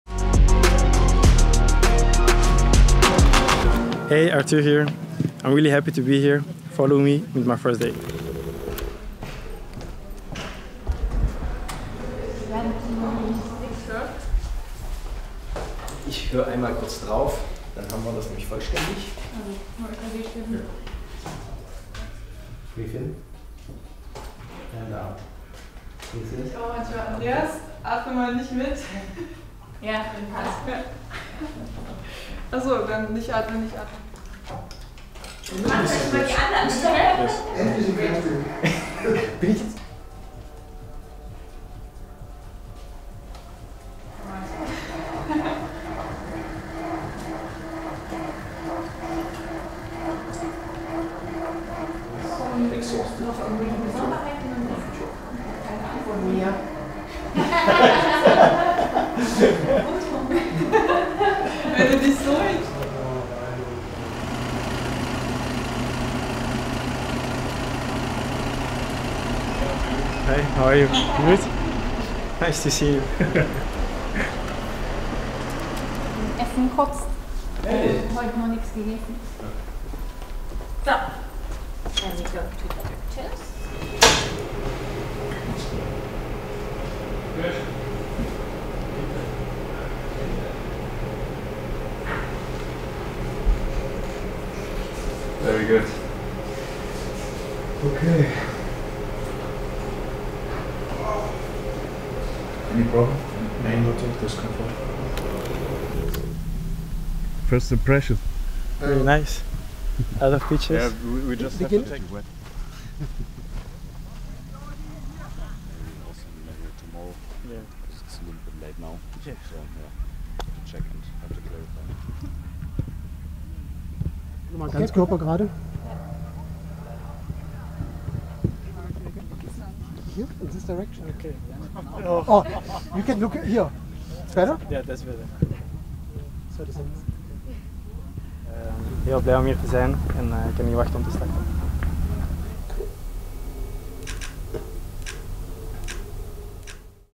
Arthur Vermeeren's first day at RB Leipzig | Behind the Scenes
Belgian new signing!

RB Leipzig sign the 19-Year-Old midfielder from Atletico Madrid. Watch his medical and his first steps in Leipzig!

Welcome to Leipzig Arthur!